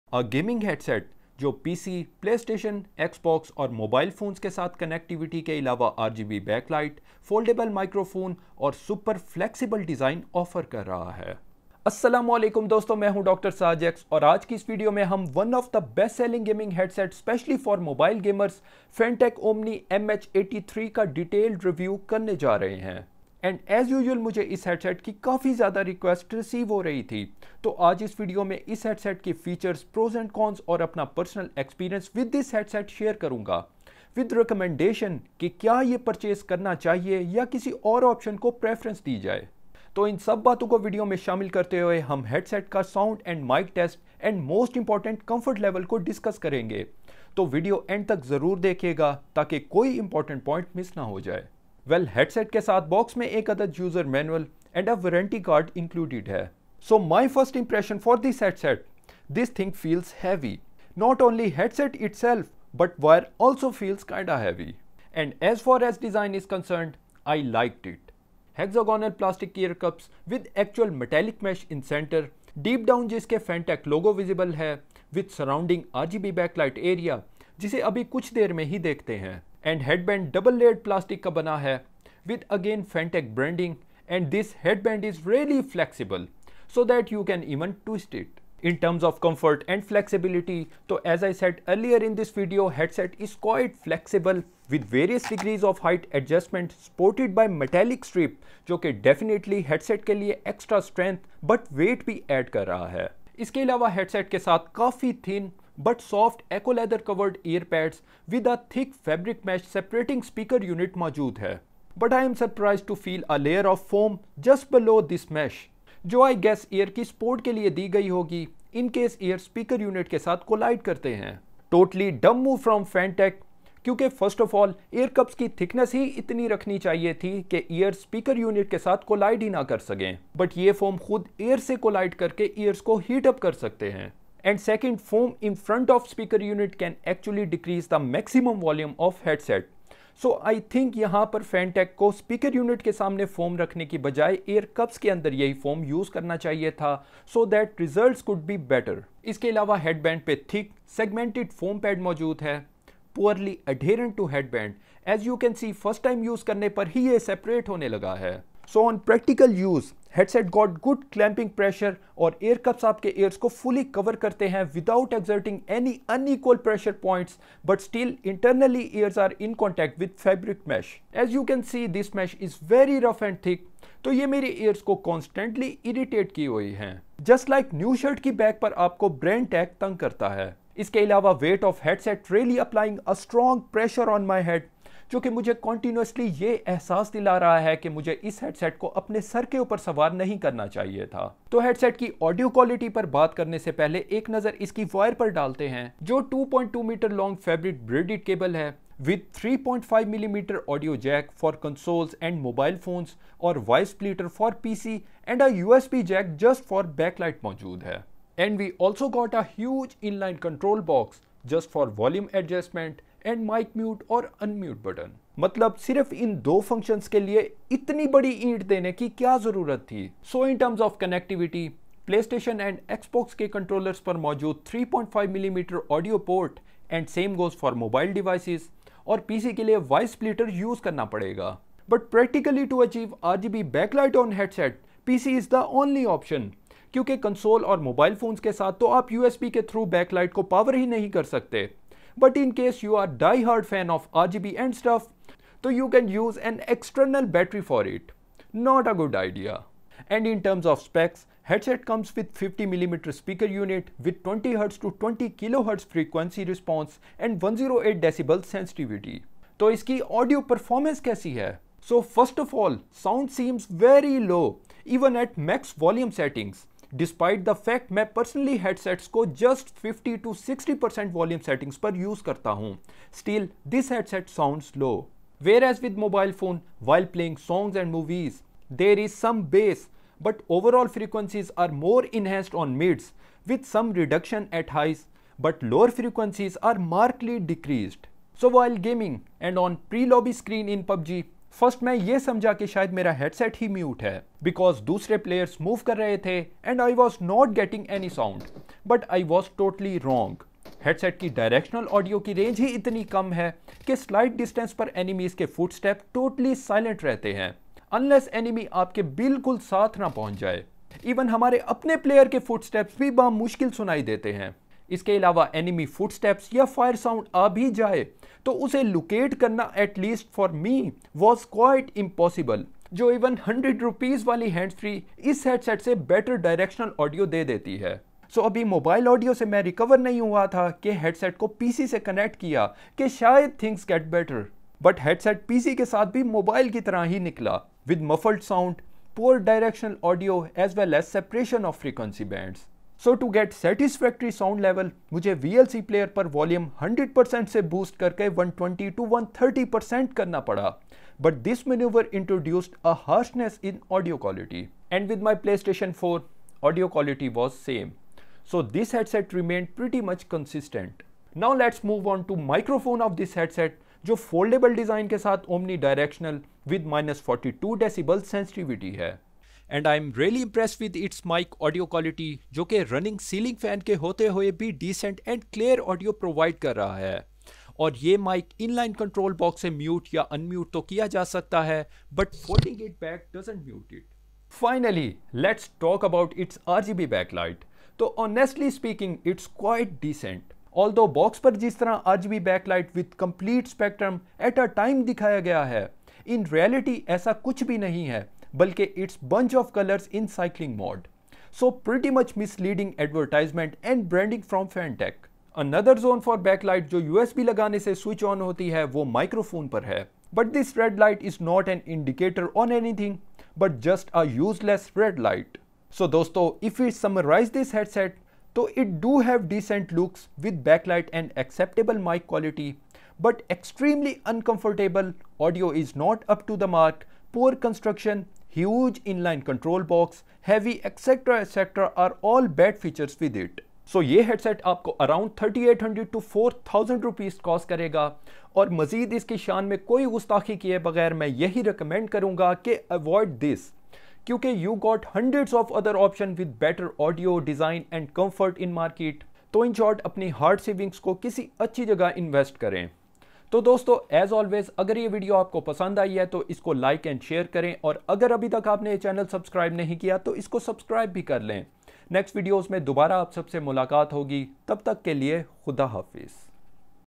STOP!!! DON'T BUY IT [ Fantech OMNI MH83 ] Multiplatform Stereo Gaming Headset with Mic | Review
Fantech OMNI MH83 Discription Details:

MULTI-PLATFORM RGB GAMING HEADSET

Designed for everywhere you game, with superior sound, comfort and style on all console gaming platforms, including PlayStation 4, Xbox One, Nintendo Switch, and mobile

Foldable Microphone:
When comes to music mode, you just swivel up microphone,
and on game mode swivel microphone down

Game & Go:
OMNI MH83 designed with foldable microphone, enjoy
your single-player game or just listen to your favorite song
Design

Fantech OMNI MH83 Tech Specs:

Channel:
Stereo 3.5mm Gaming Headset

Illumination:
RGB Lighting

Remote Control:
In-line Remote

Core Construction:
ABS Plastic + Alluminium

Weight:
315gr (without cable)

Connector Type:
TRRS + USB

Cable:
2.2mm Nylon braided cable

Detachable Cable:
No

Detachable Microphone:
No

In this video we have done detailed and honest review of one of the most selling multi platform gaming headset with mic works with Xbox , playstation , PC and mobile phones that is Fantech OMNI MH83 but Stop! before you buy this headset, must watch our this review so that you don't buy a product not worth you money. Note headset do got a good noise cancellation mic !